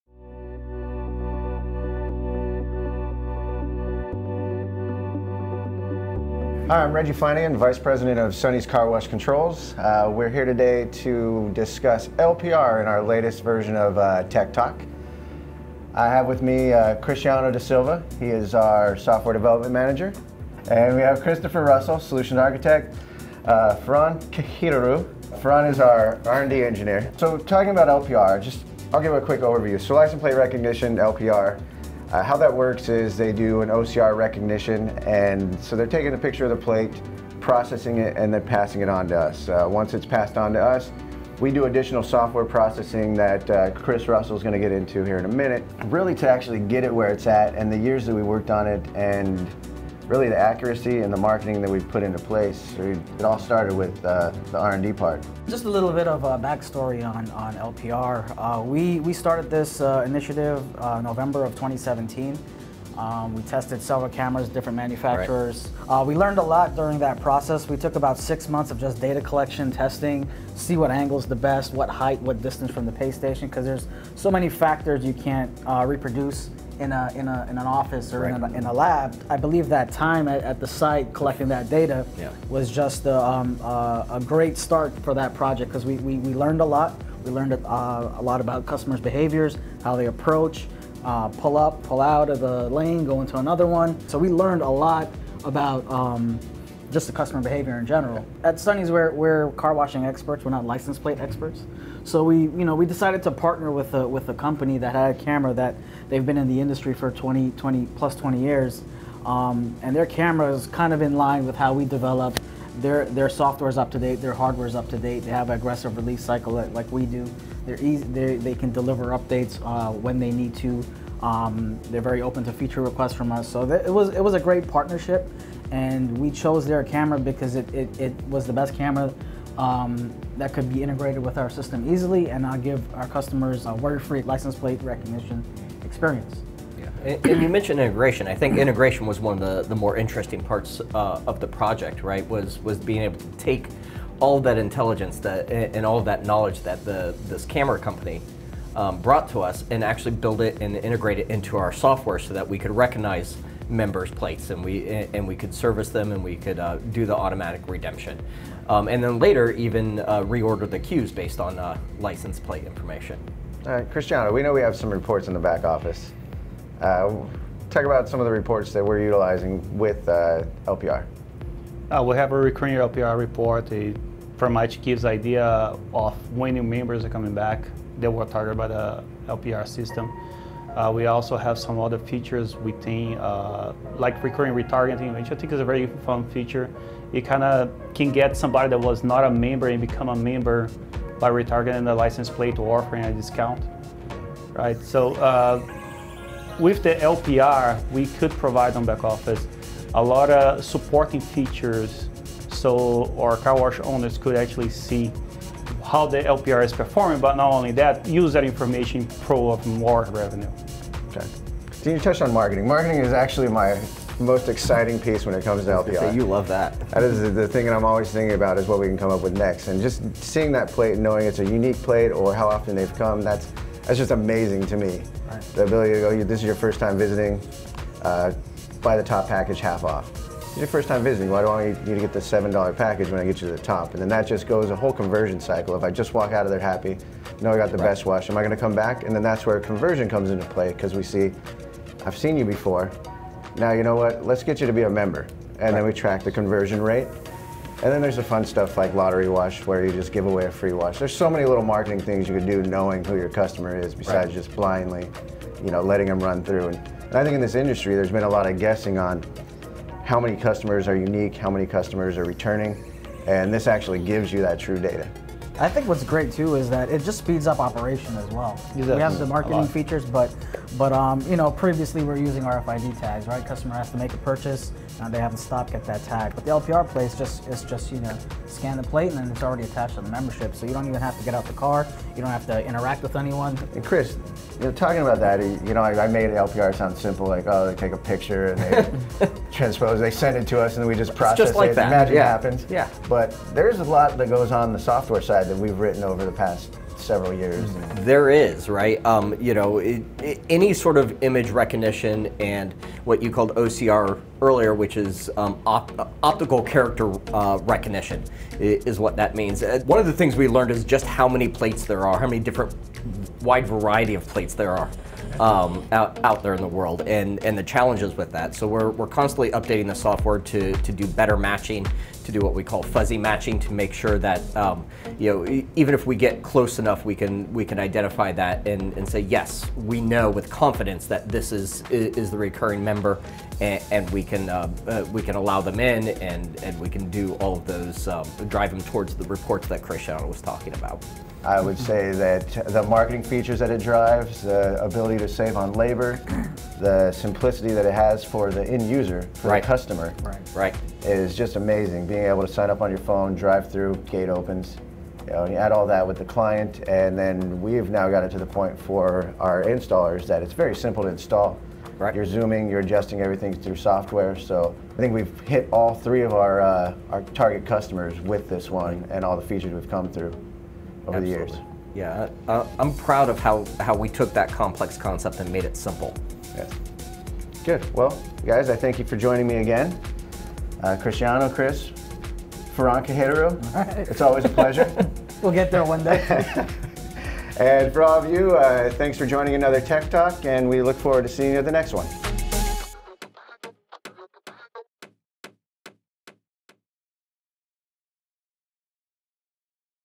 Leveraging License Plate Recognition | Sonny's CarWash Controls Tech Talk
In this episode, we discuss how Sonny's CarWash Controls team conceived the idea of License Plate Recognition (LPR) to better automate car wash lines, and move the car wash industry into a new chapter of efficiency. Learn how LPR helps operators increase revenue, provides convenient scan-and-go capabilities and contactless registration, enhances the customer experience, and makes operations more efficient.

Leveraging License Plate Recognition
0:30 - Optical Character Recognition
0:55 - LPR Background
3:00 - LPR Features
4:55 - Future Enhancements
5:40 - LPR Benefits